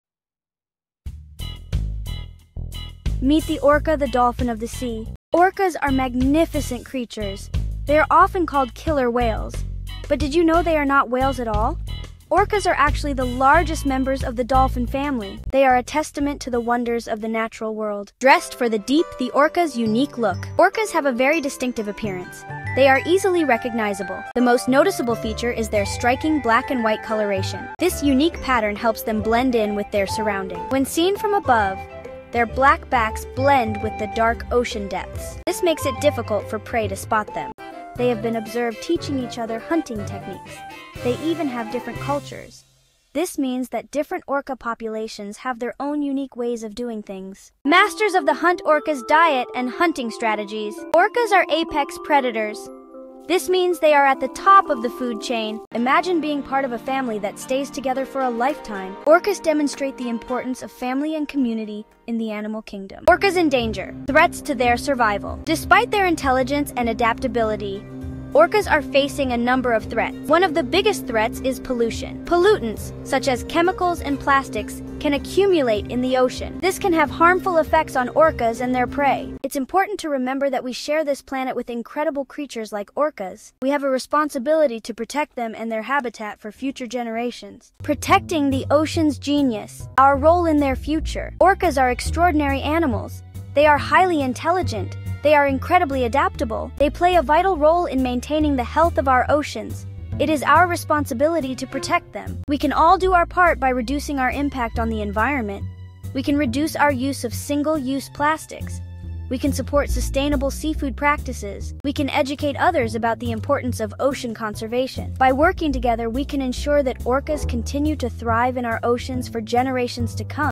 Orcas: Dolphins or Whales? You Won't Believe The Truth!
Dive into the world of orcas, the ocean's majestic dolphins! Explore their unique appearance, hunting strategies, and the importance of family. Discover the threats they face and learn how we can protect these incredible creatures for future generations. Join us in preserving the ocean's genius!